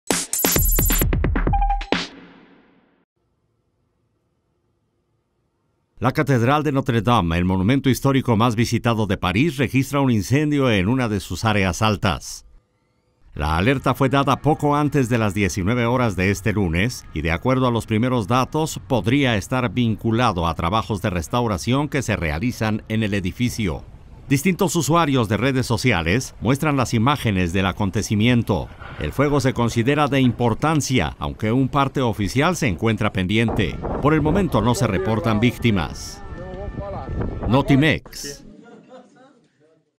Catedral de Notre Dame registra incendio
La alerta fue dada poco antes de las 19:00 horas de este lunes, y de acuerdo a los primeros datos, podría estar vinculado a trabajos de restauración que se realizan en el edificio.